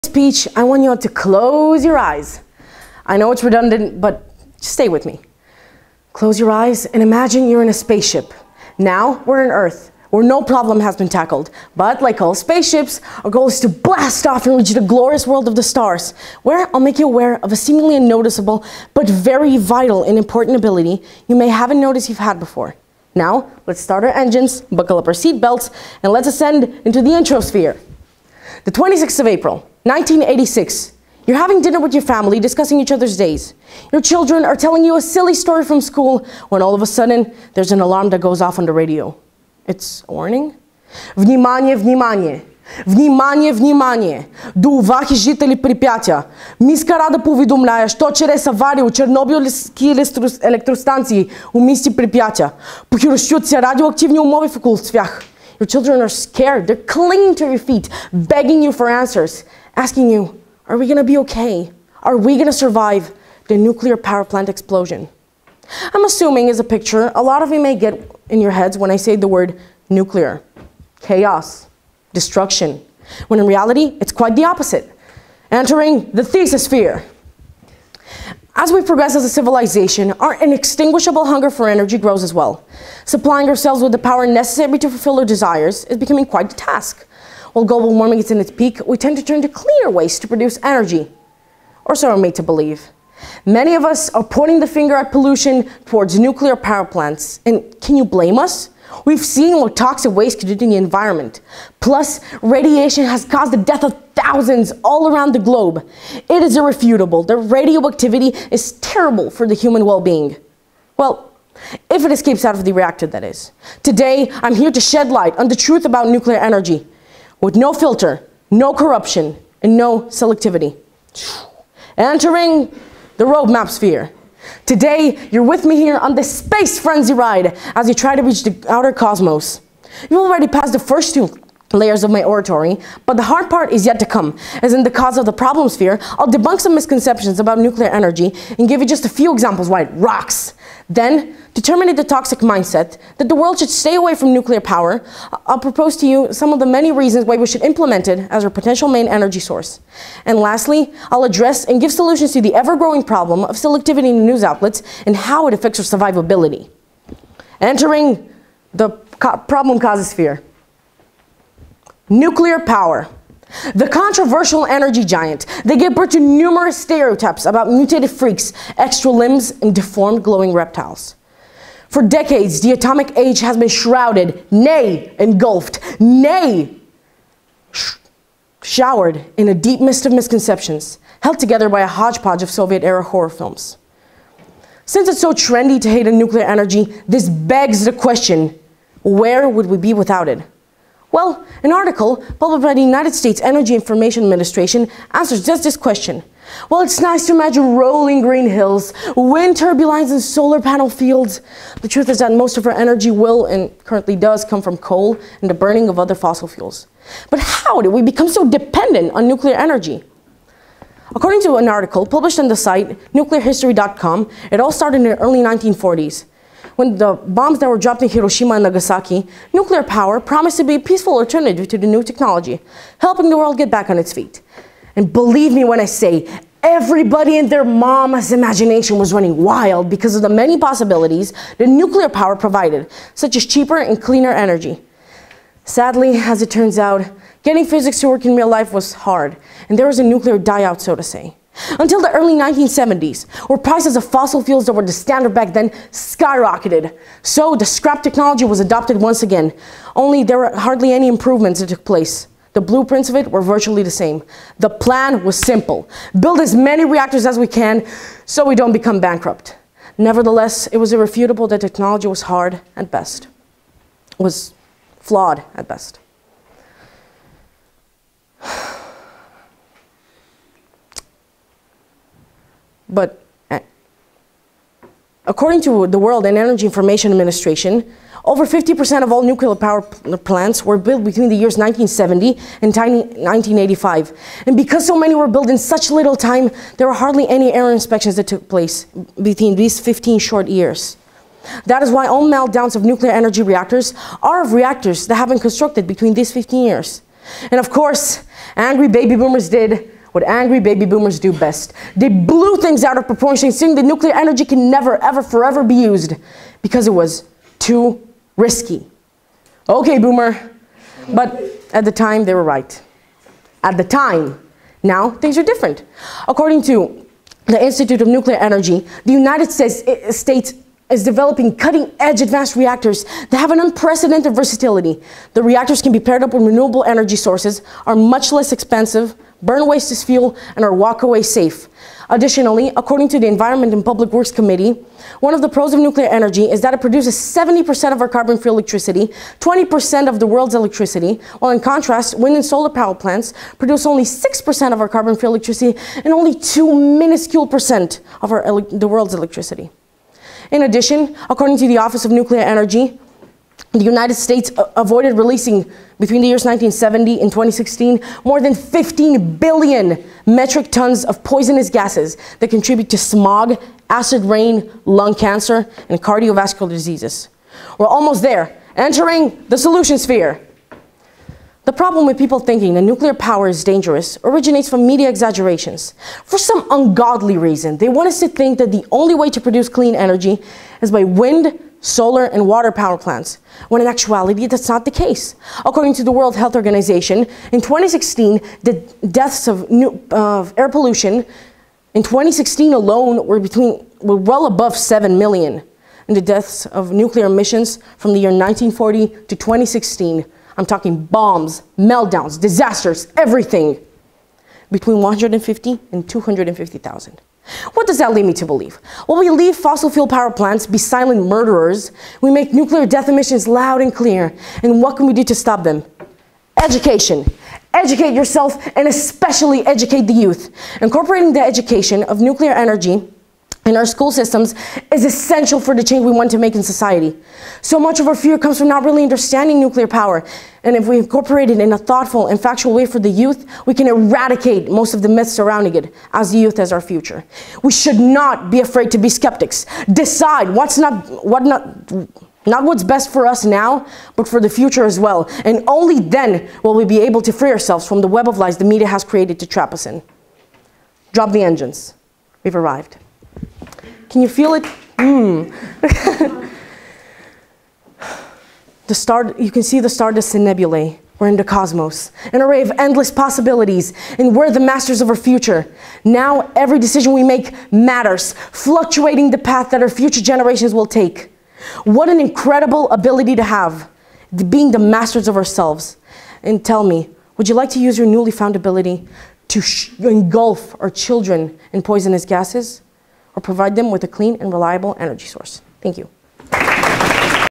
Stephan presents “Nuclear Energy: a blessing or a curse?”
Stephan Kozelov presents his own work, “Nuclear Energy: a blessing or a curse?”
Original Oratory, Fall West Regional Tournament, Sofia, 2019